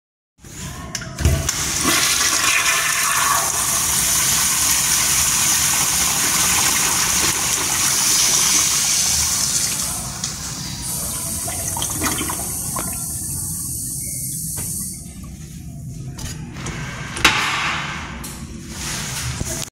1980's Eljer Newark 3!
This toilet is awesome! It has an awesome flush!